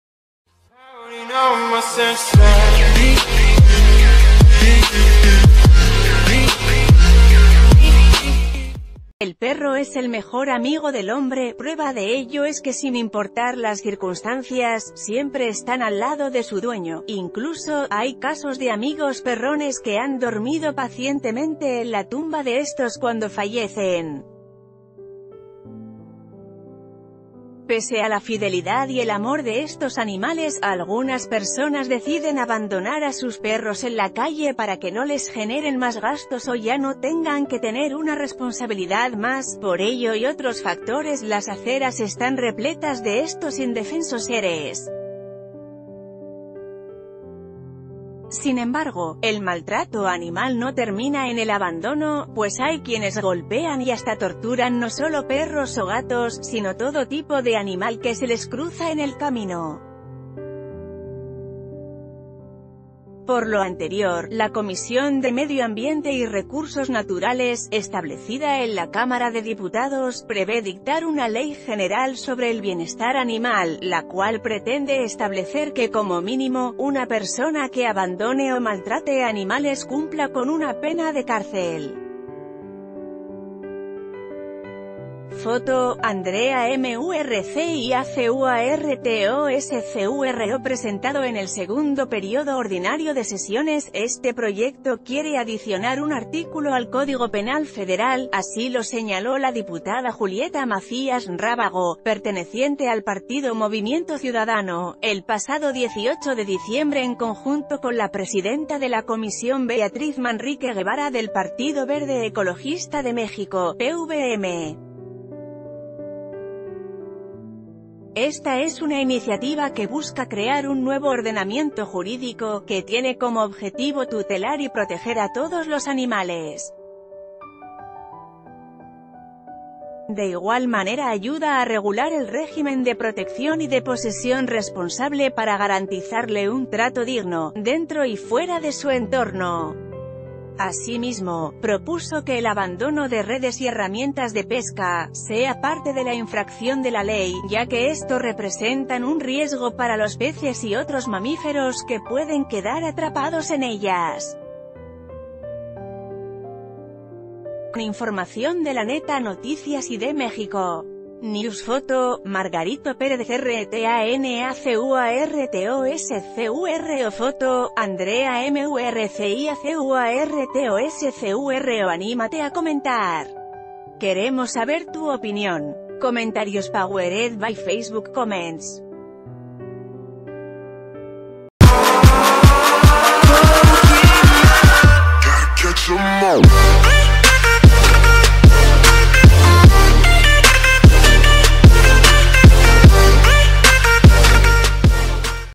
Diputados van por castigar con cárcel el abandono y maltrato animal
El perro es el mejor amigo del hombre, prueba de ello es que sin importar las circunstancias, siempre están al lado de su dueño; incluso, hay casos de ‘amigos perrones’ que han dormido pacientemente en la tumba de estos cuando fallecen. Pese a la fidelidad y el amor de estos animales, algunas person...